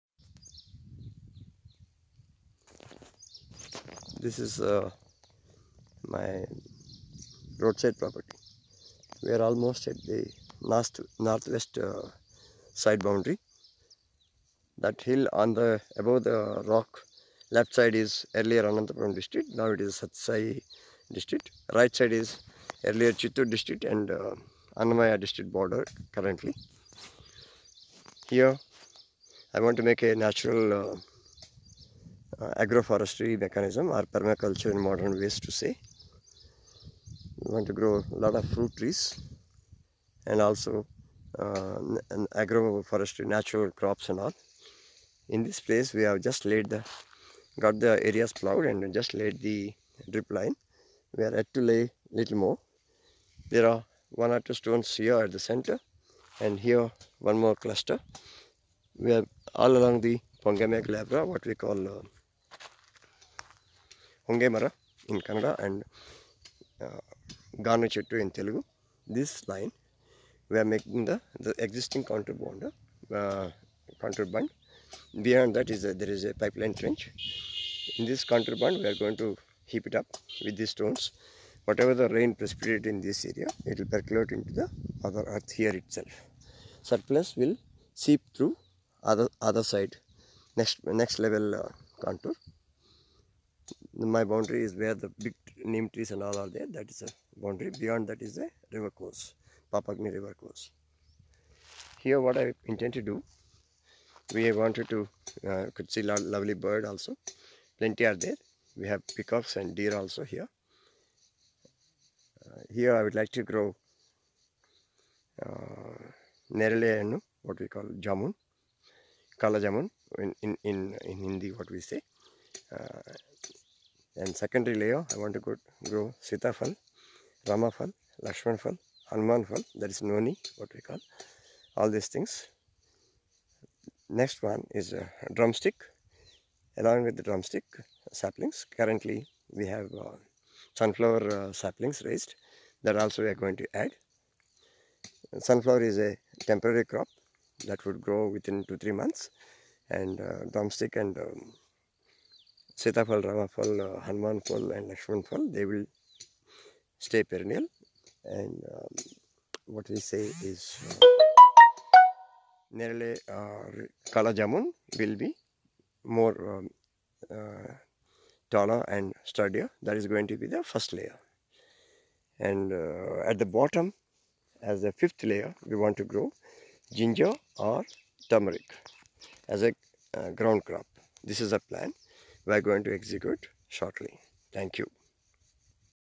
Multiple Layered Fruit Orchards
Intended to Grow for Home / Personal use:
* Jamun (Neryedu / Nerale Hannu),
* Drumstick
* Ramaphal
* Seethaphala (Custard apple)
* Lakshman phala (Sour)
*  Hanuman phala (Noni) etc.
* Aloe vera;
* Turmeric / Ginger
As Ground cover Crops